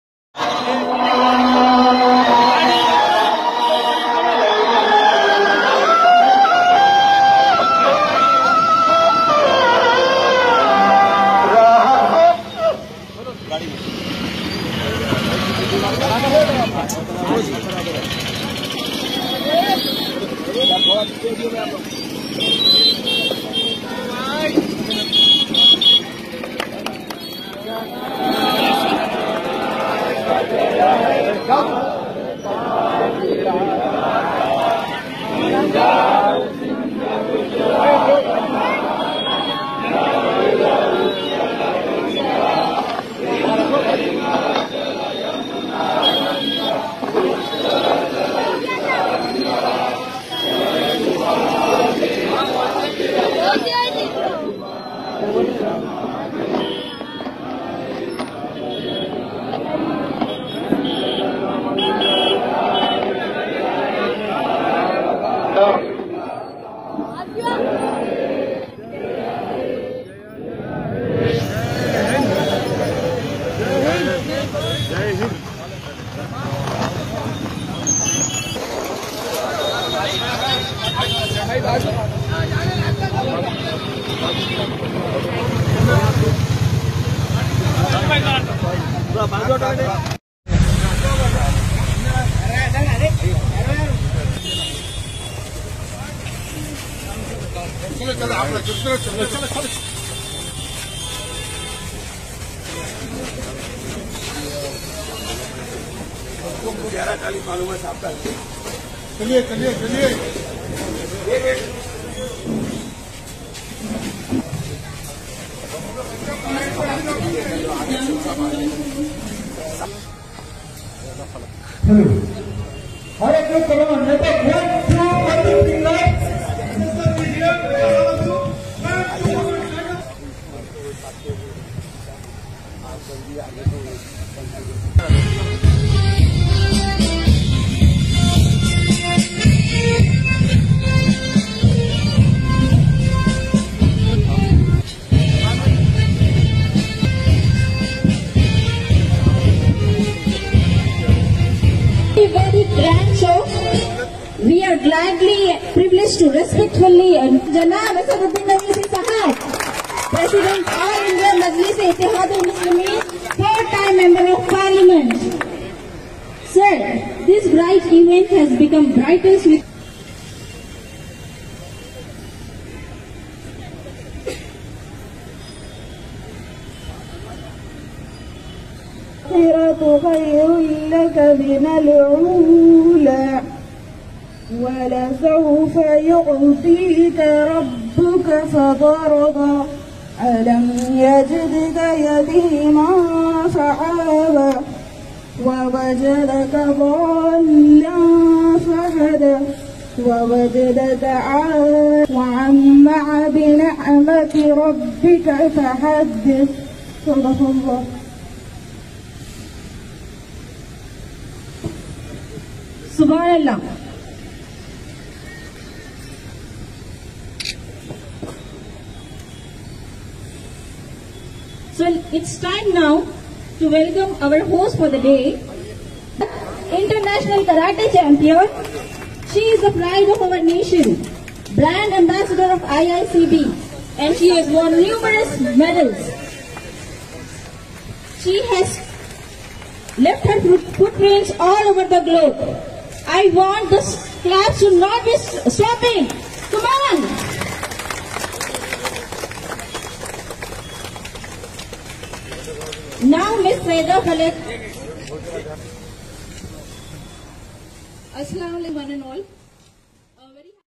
15 August, asaduddin owaisi Celebrate independence day, bike ride & flag | AIMIM News Heydrabad
15 August, asaduddin owaisi Celebrate independence day, bike ride & flag | AIMIM News Heydrabad